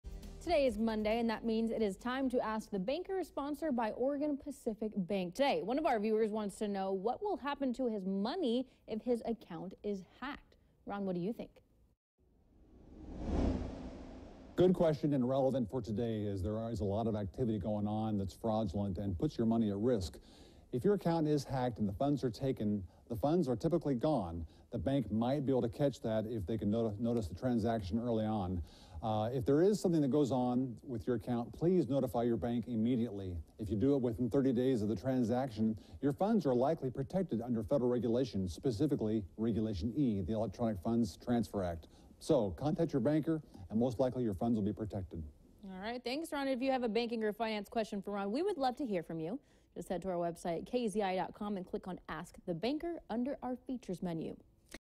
Ask The Banker: What to do if your account gets hacked
KEZI talked to Ron Green with Oregon Pacific Bank.